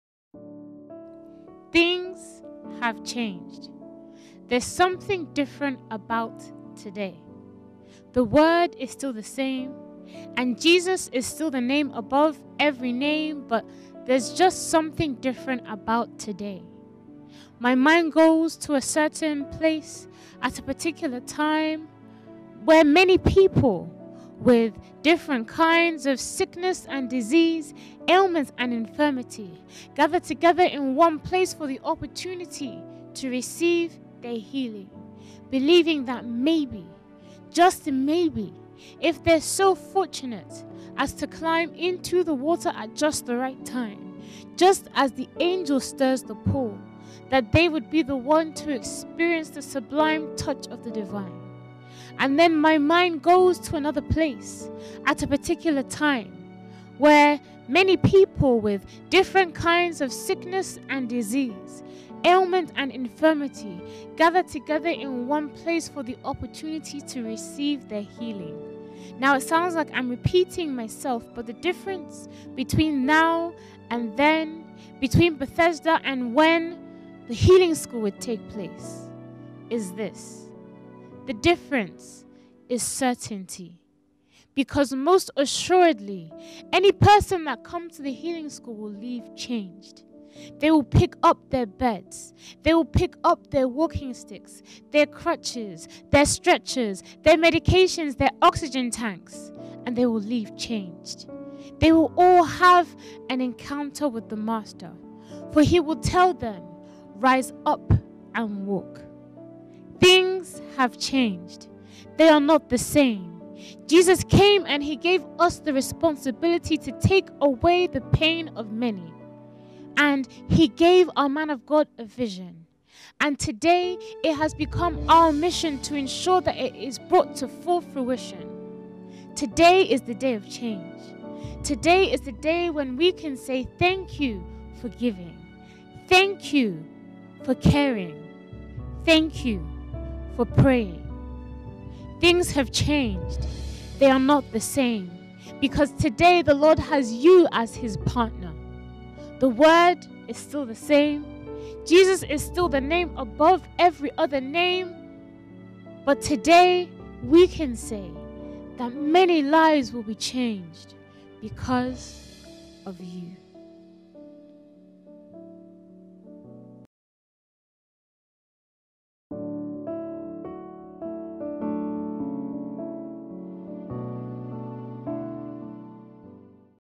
Things Have Changed - A Spoken Word Piece
This piece was recorded for the Healing School Prayer Conference, which took place online in July 2019. It compares the Healing School with the pool of Bethesda as mentioned in the Bible and parallels the experience of the lame man with Jesus with experience of the students after the Healing School. It is a thank you tribute to all the partners that make the Healing School possible.

To learn more about the Healing School, go to enterthehealingschool.org.

Credits:
Video Recording - Arnold Nyatwa
Instrumental - The Platform Group